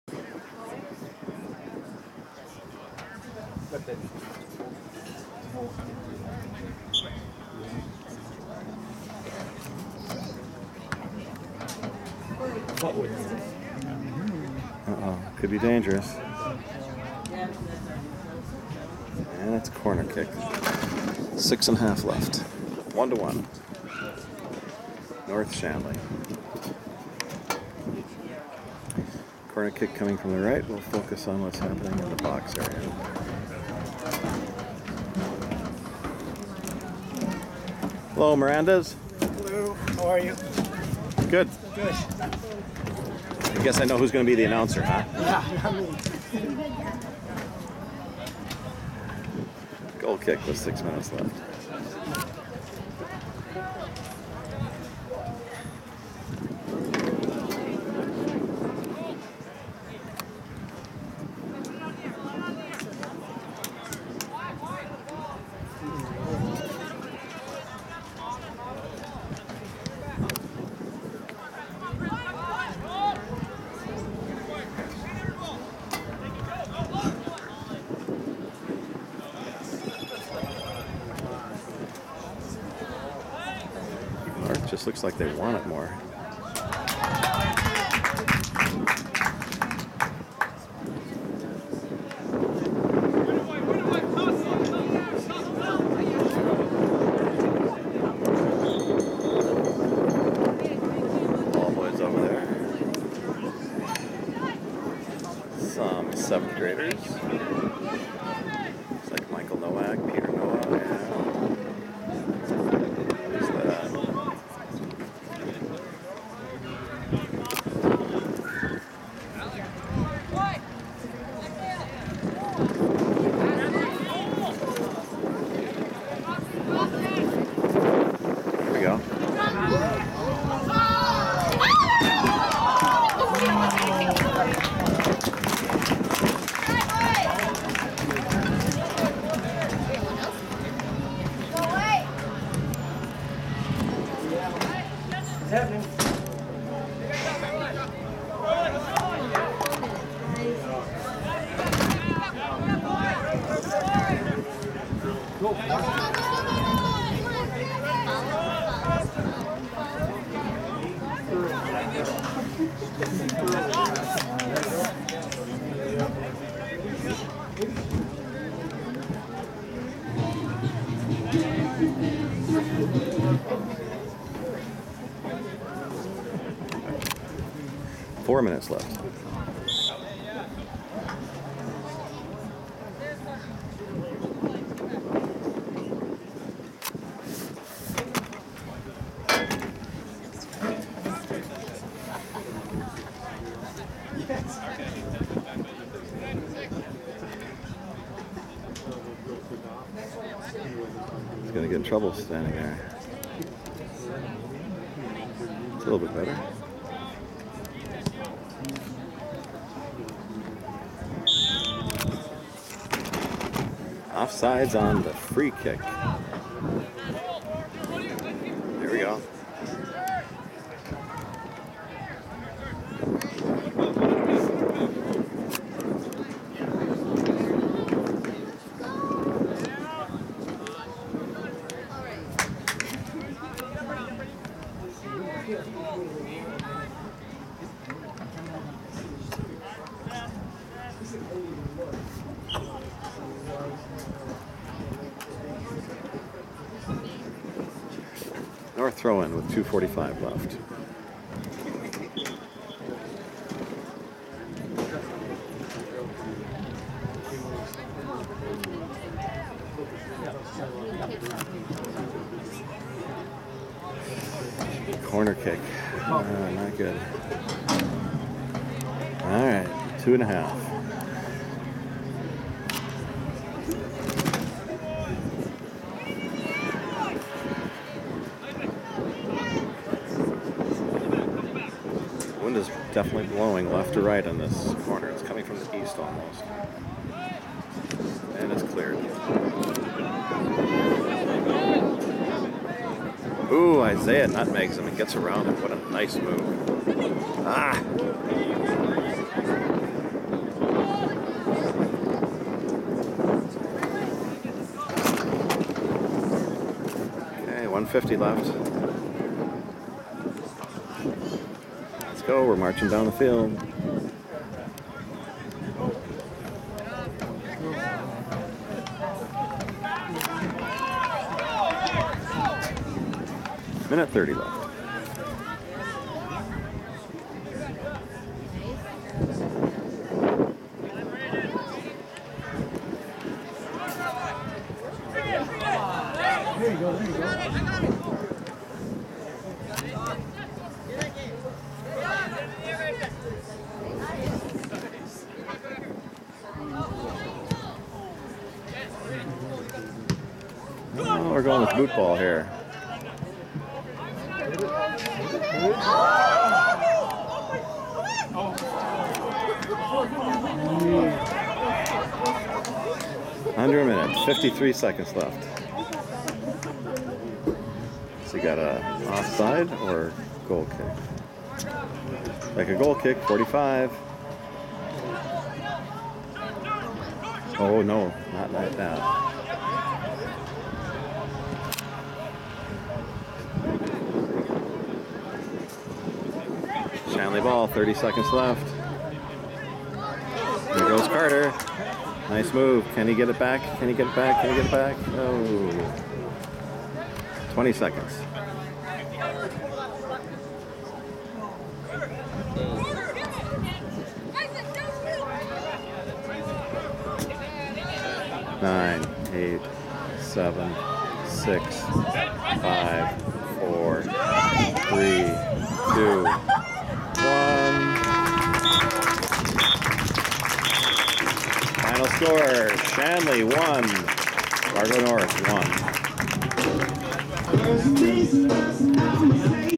2018 09 04 Shanley v North JV 6 of 6
September 4, 1028.  Fargo Shanley boys JV soccer vs. Fargo North JV.  The game ended in a 1-1 tie.